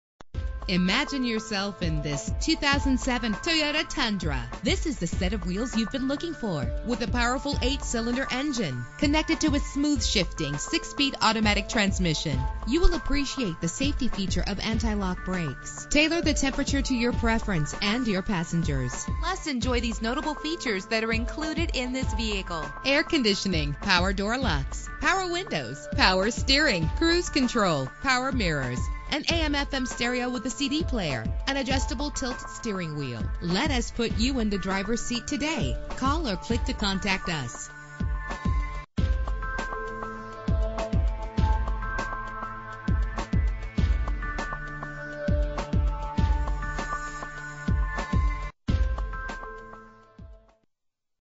2007 Toyota Tundra - Memphis TN
Year: 2007
Make: Toyota
Model: Tundra
Trim: SR5
Engine: 5.7 liter 8 cylinder 32 valve MPFI DOHC
Transmission: 6-Speed Automatic
Color: RO
Mileage: 69001
Address:
7727 US Highway 64